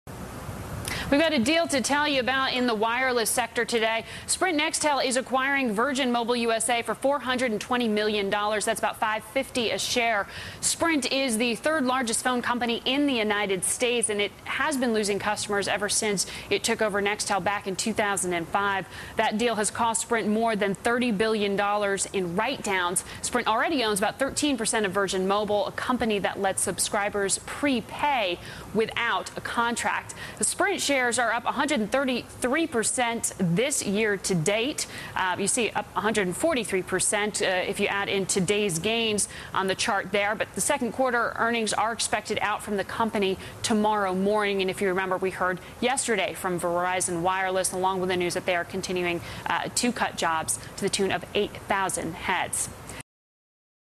Sprint To Acquire Virgin Mobile USA - Bloomberg
Sprint acquires Virgin Mobile for $420 million. (Bloomberg News)